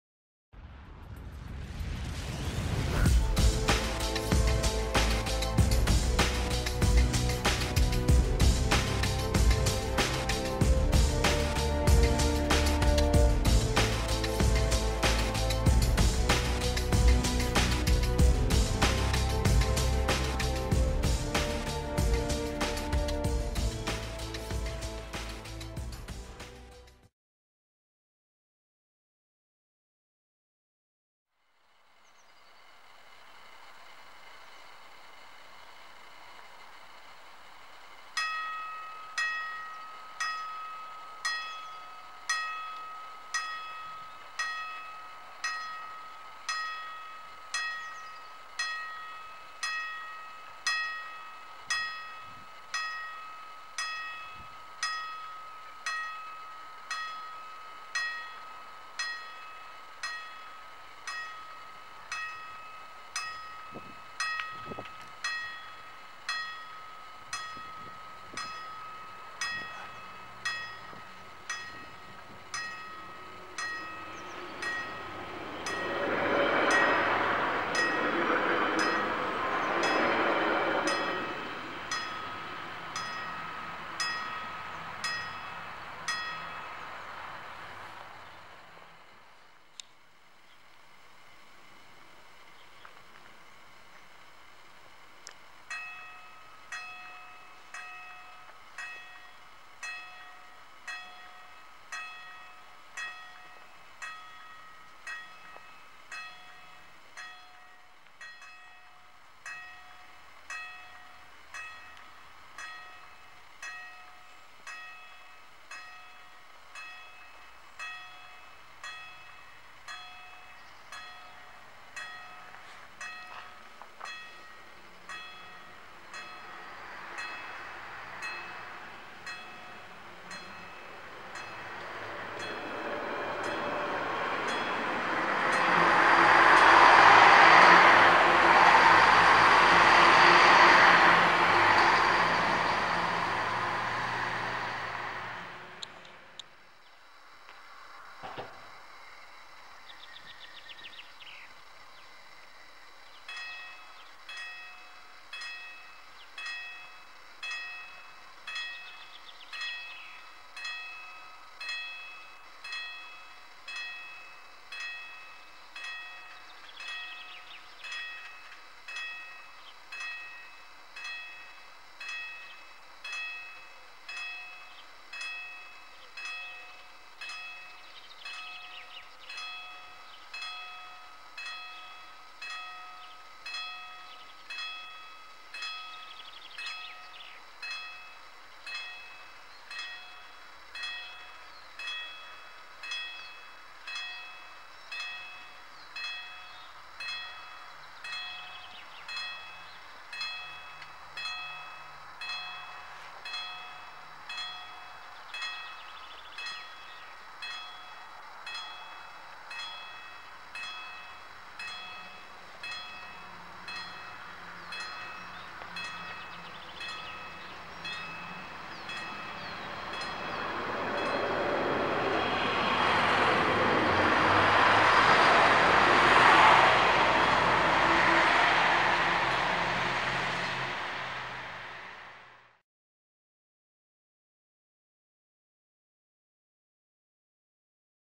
Bahnübergang Windsbach "Raiffeisenstraße"
Windsbach ist eine Kleinstadt im Landkreis Ansbach und bildet den Endpunkt der von Wicklesgreuth ausgehenden Bahnstrecke, auch "Löhe-Express" genannt. Direkt vor dem Haltepunkt liegt in der Raiffeisenstraße einer der wenigen technisch gesicherten BÜ dieser Strecke. Immerhin machen die zahlreichen Pfeiftafeln ein rechtzeitiges Einschalten der Kamera möglich, zumindest bei der Einfahrt in den Haltepunkt. Der Bahnübergang verfügt über Blinklichter von Pintsch und kommt ohne Schranken aus. Interessanterweise ist der Wecker nicht an einem der Lichtmasten befestigt, sondern am Schalthäuschen, wie in der ersten Aufnahme gut zu sehen ist.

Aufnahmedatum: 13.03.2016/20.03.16